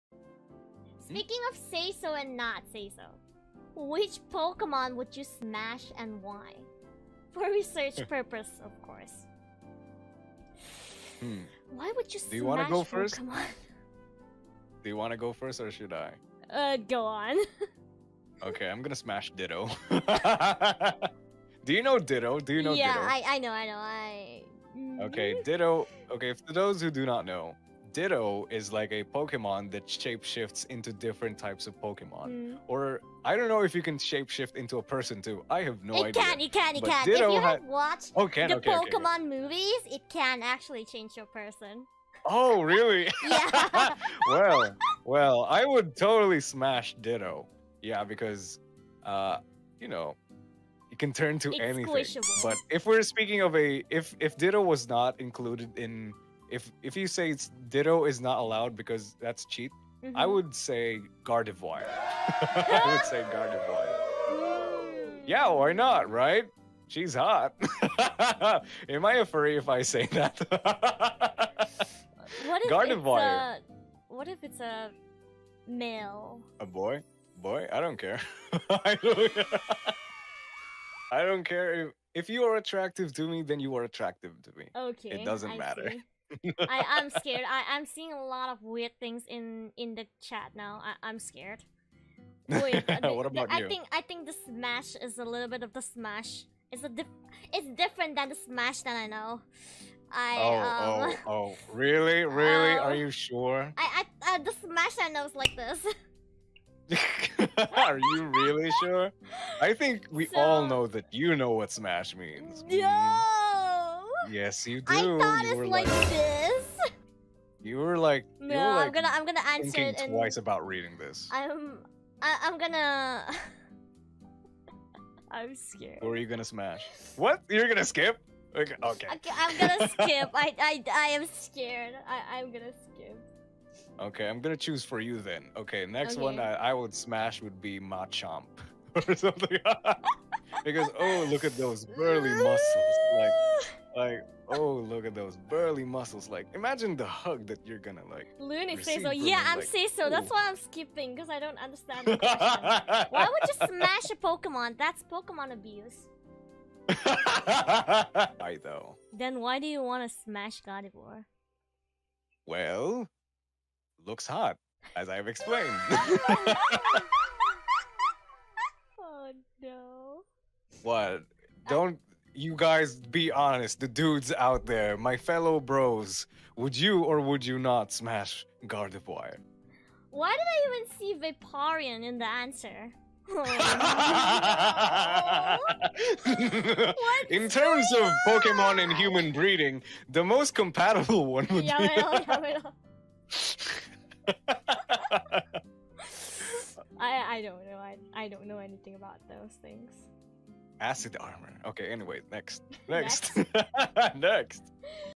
Objectively sexy pokemon
We all know Vaporeon is the sexiest pokemon

The two featured VTubers in this video.

Thumbnail made by: Kay